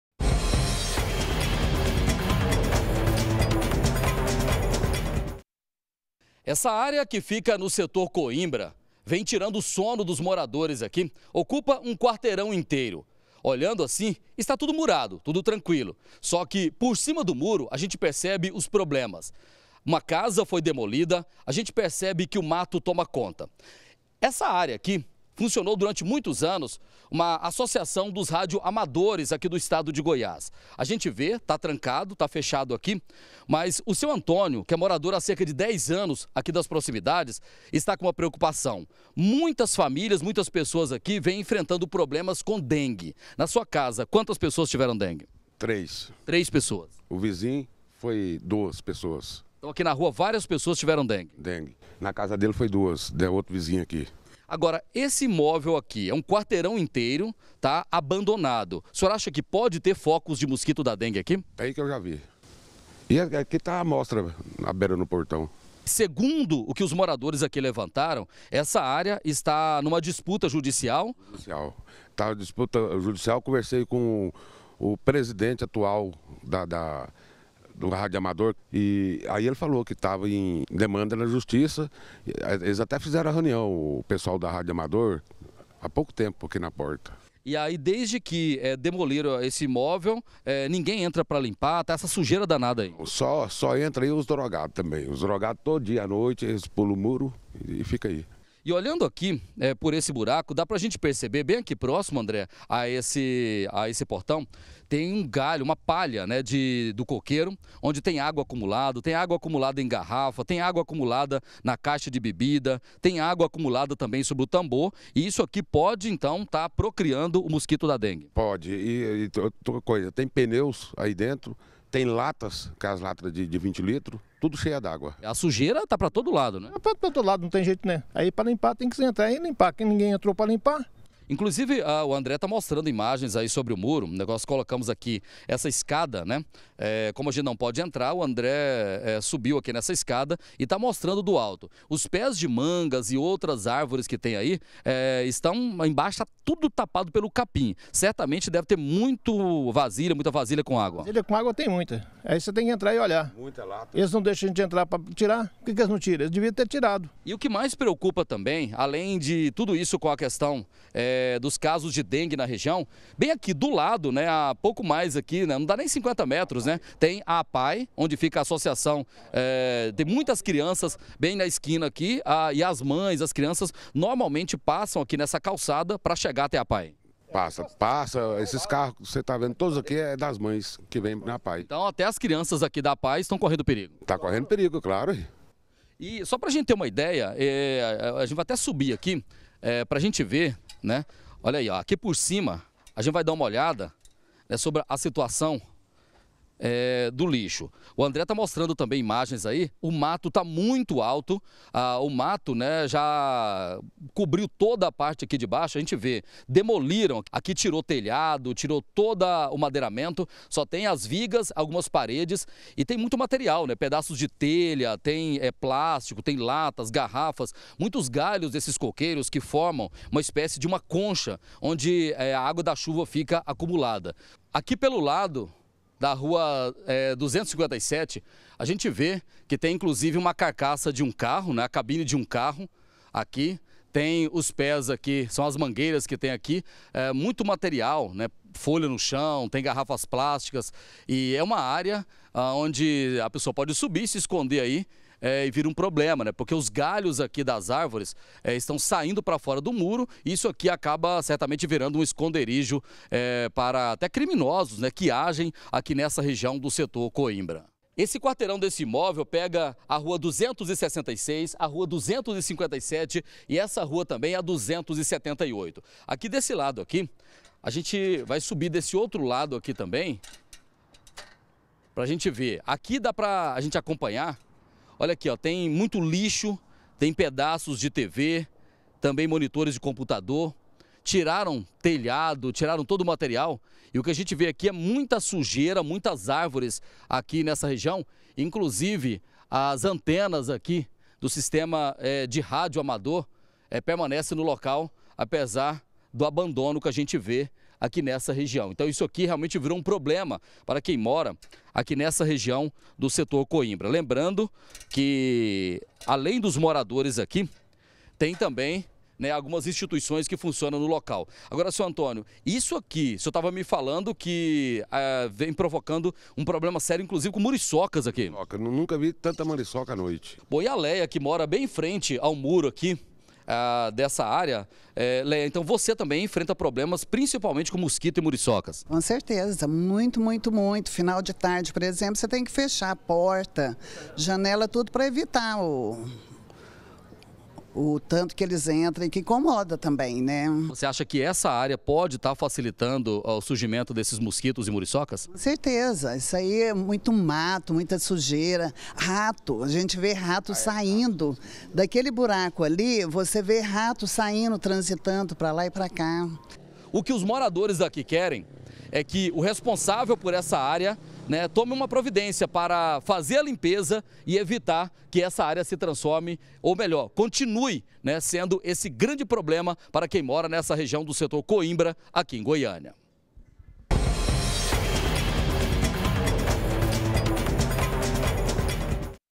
BG - Dengue: Área abandonada preocupa - 22-03-2018
Área abandonada, no Setor Coimbra, preocupa moradores por causa da dengue.